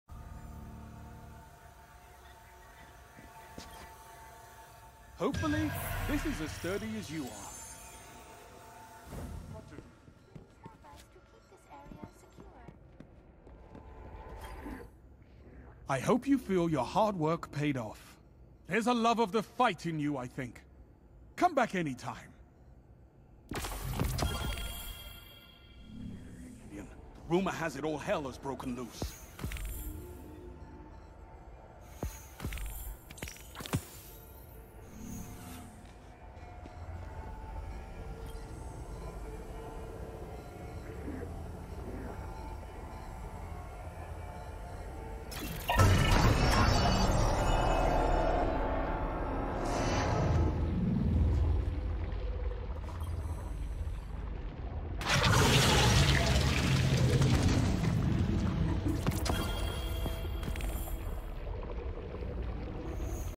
Unlocking The Luna Howl Handcannon and The Superblack Shader Feels Good!
I disliked the grind to unlock all of the Brave weapons, its over finally, woohoo!

Hello superblack! How are you?

GG!